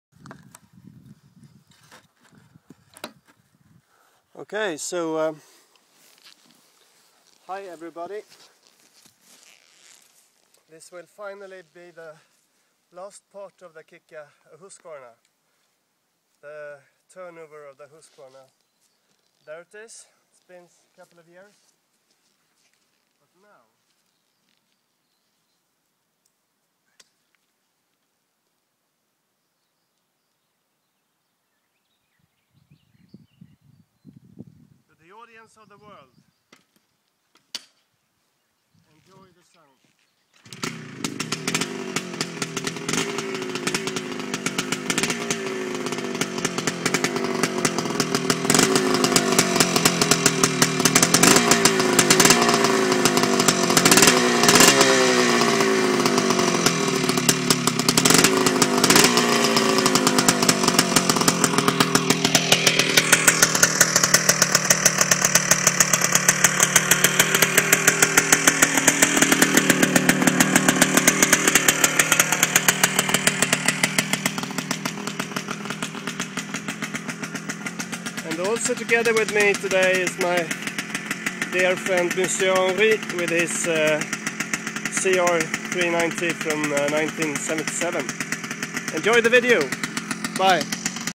Kicka en Husqvarna 5 - sista episoden (The turn Over of the Husqvarna - Last episode)
Så hände det till sist. Min gamla Husqvarna 250cc från 1968 gick igång... och som den gick sedan. Nyrustad med Femsa, packboxar, portad, tät som en plastpåse och med upplagt avgassystem från en 360. Bakhjulsåka när det är som bäst. Den andra hojen är en tävlingspreppad cr 390 från 1977 som min kompis brukar brottas med.
So, finely it happend. My old Husky 250cc from 1968 came to life! And what a life! Restored with a Femsa flywheel, sealings, blue printed and with a exhaust from a 360. The other bike is a racing equipped cr 390 from 1977. My friend is usually wrestling around with that beast.